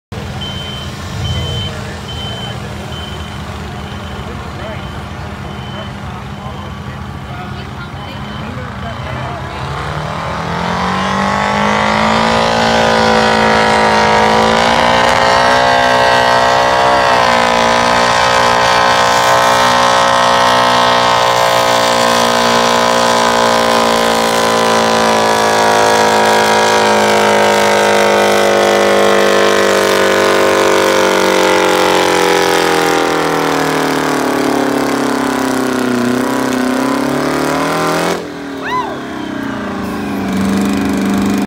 Oliver 1950 4-53 Detroit 7,000lb KITPA New Castle, KY 3rd place
Our first hook of the year for 2018. Came home with a 3rd place finish in the KITPA 7,000lb farm stock class in New Castle, Kentucky. May 11, 2018